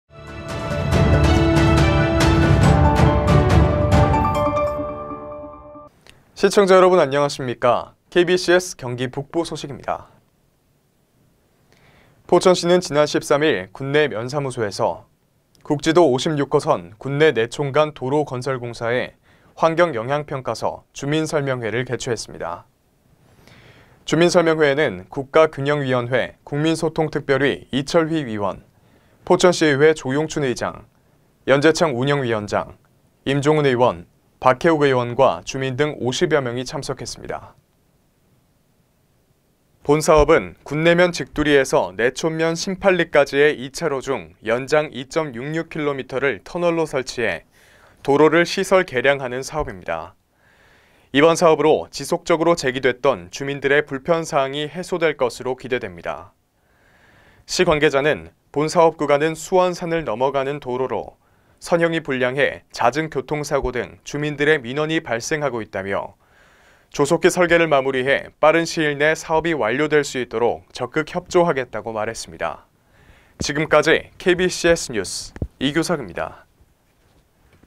포천시, 환경영향평가서 주민설명회 열어
포천시는 지난 13일 군내면사무소에서 국지도 56호선 「군내~내촌(수원산터널)도로건설공사」의 환경영향평가서(초안) 주민설명회를 개최했다.

주민설명회에는 국가균형위원회 국민소통특별위 이철휘 위원, 포천시의회 조용춘 의장, 연제창 운영위원장, 임종훈 의원, 박혜옥 의원과 주민 등 50여 명이 참석했다.

본 사업은 군내면 직두리에서 내촌면 신팔리까지의 2차로 중 연장 2.66km(총 연장 5.5km)를 터널로 설치하여 도로를 시설 개량하는 사업이다. 이번 사업으로 지속적으로 제기되었던 주민들의 불편사항이 해소될 것으로 기대된다.

시 관계자는 “본 사업구간은 수원산을 넘어가는 도로로, 선형이 불량해 잦은 교통사고 등 주민들의 민원이 발생하고 있다.”라며 “조속히 설계를 마무리해 빠른 시일 내 사업이 완료될 수 있도록 적극 협조하겠다.”라고 말했다.